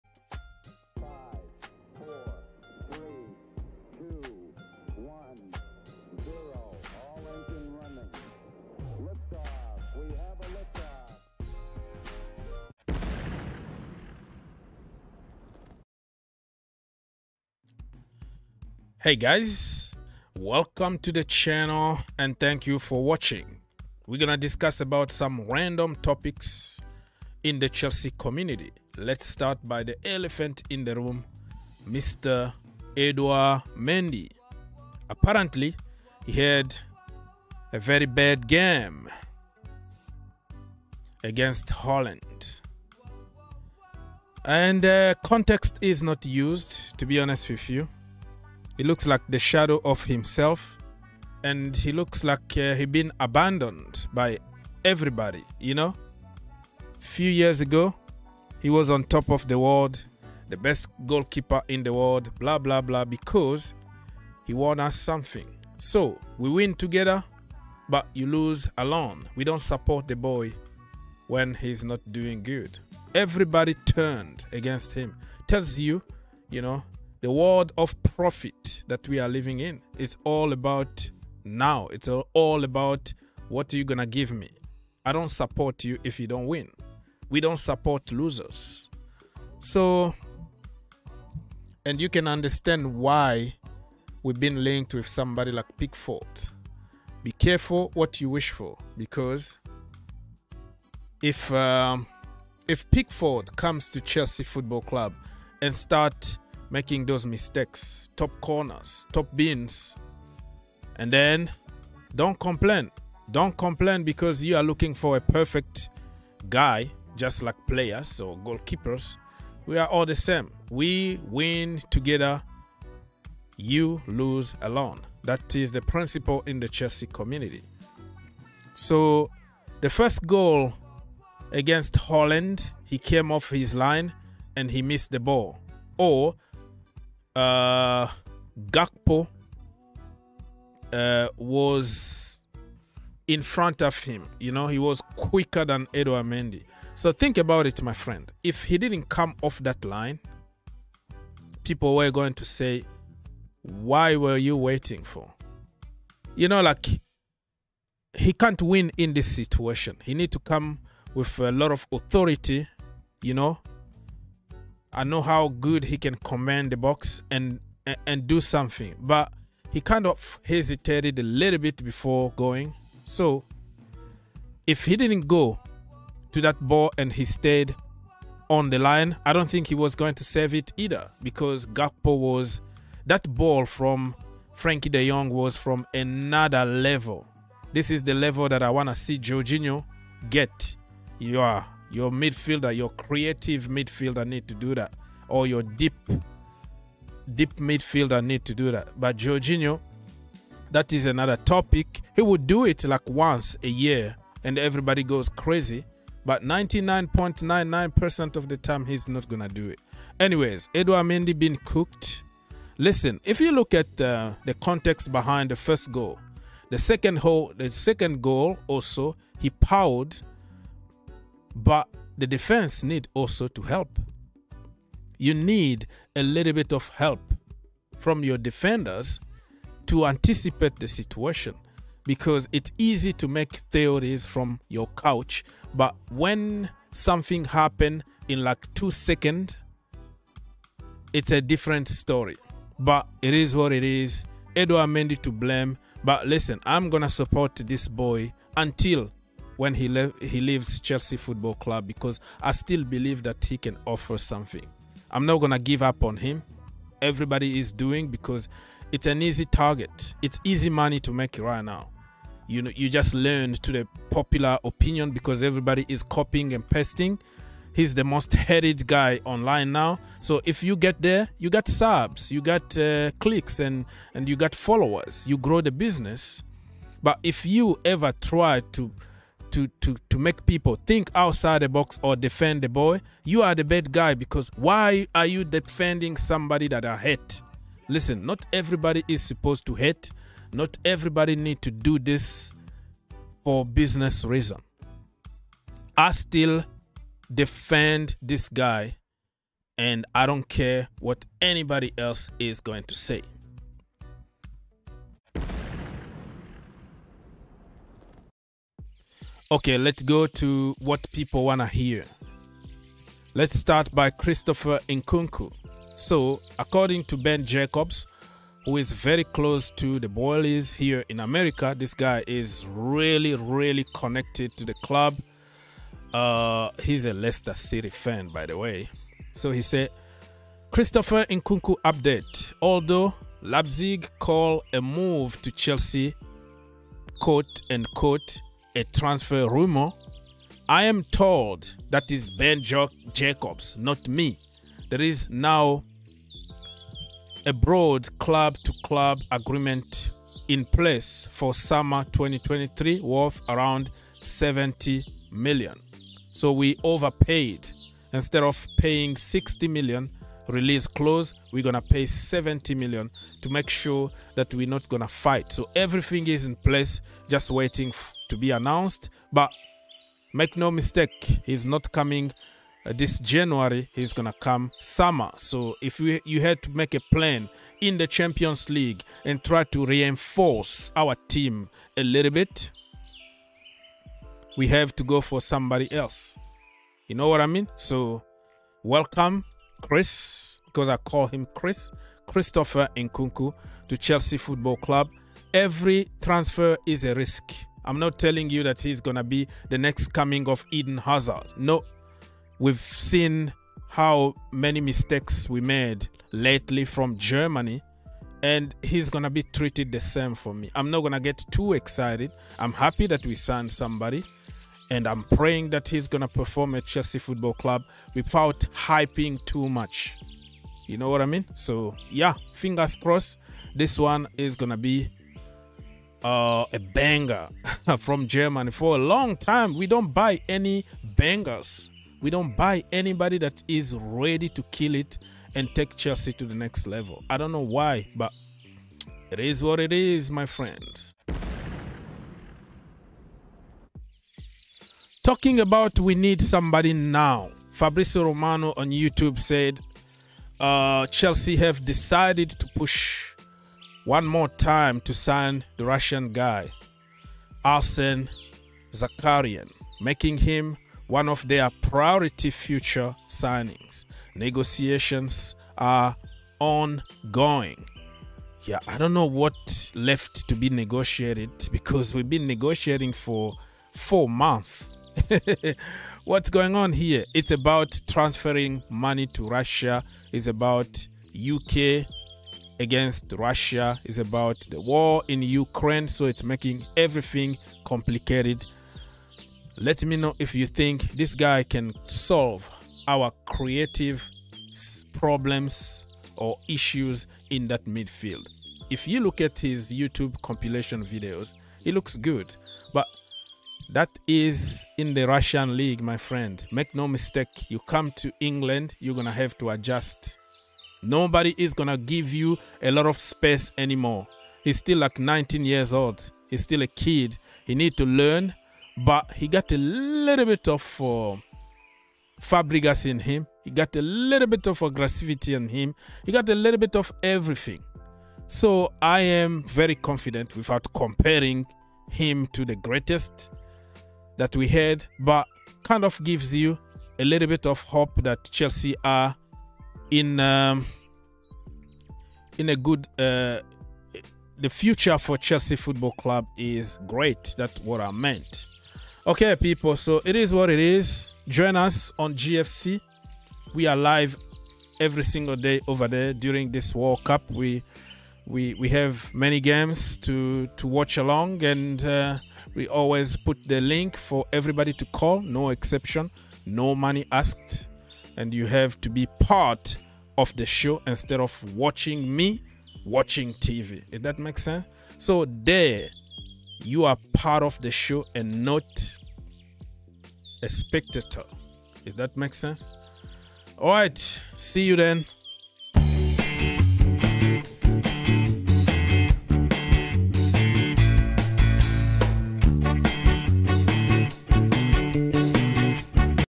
Nkunku Deal DONE! Edouard Mendy Scapegoated | Zakharyan - Chelsea Transfer Updates
Chelsea Latest Transfer News
Christopher Nkunku to Chelsea is DONE!
Edouard Mendy Horror Show vs Netherlands
Arsen Zakharyan closing on Chelsea